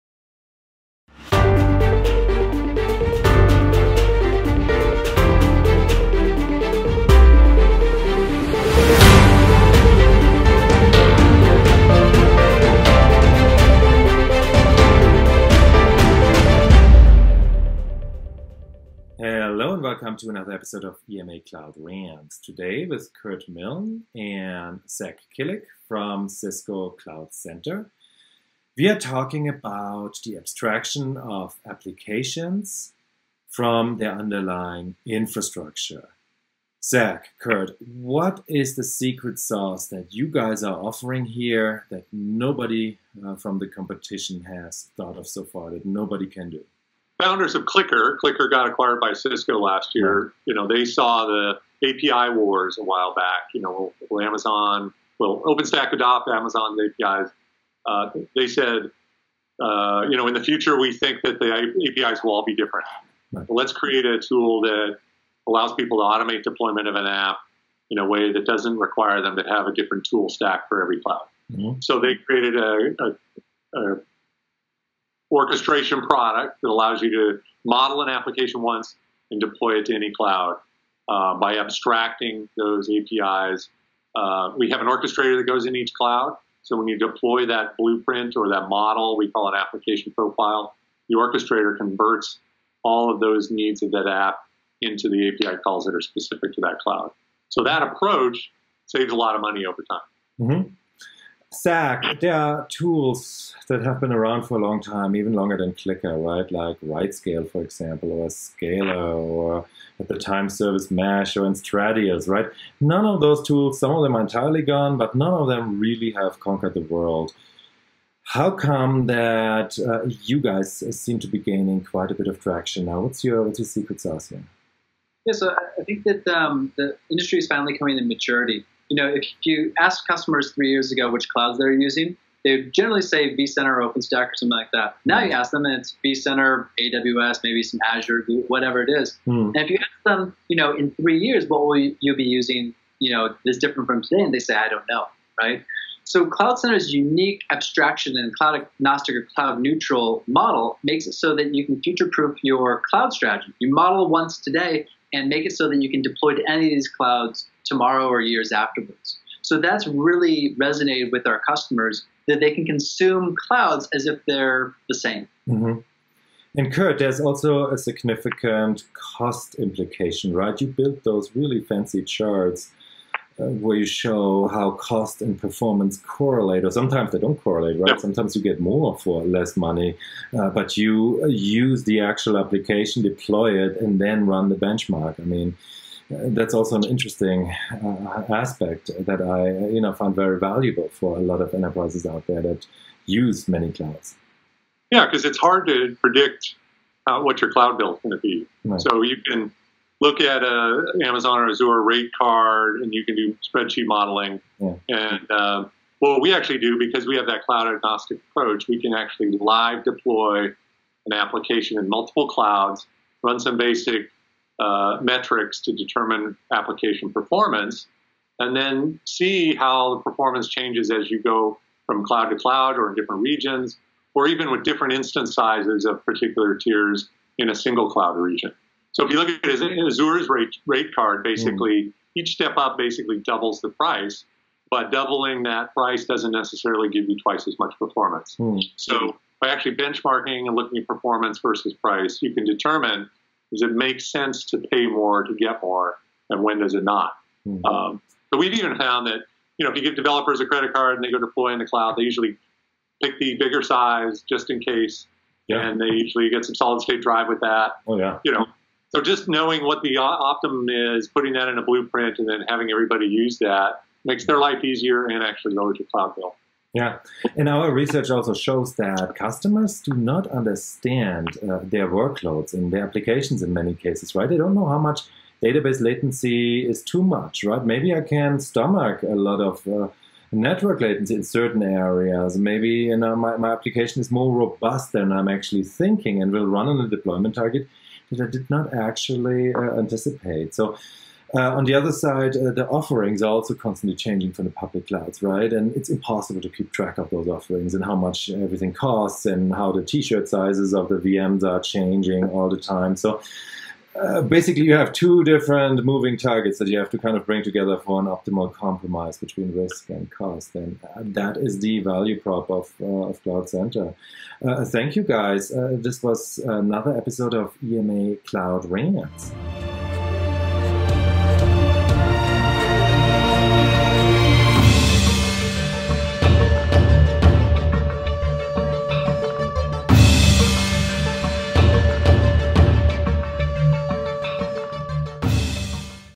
EMA Cloud Rants - Cisco CloudCenter
Cisco CloudCenter received the EMA Top 3 award for its ability to optimize cost across public clouds and the corporate data center. Kurt Milne (Cisco, VP Product Marketing) and Zack Kielich (Cisco, Product Manager) discuss with Torsten how Cisco's approach to infrastructure-independent application deployment and day 2 management changes the cost game in a more and more heterogeneous cloud game.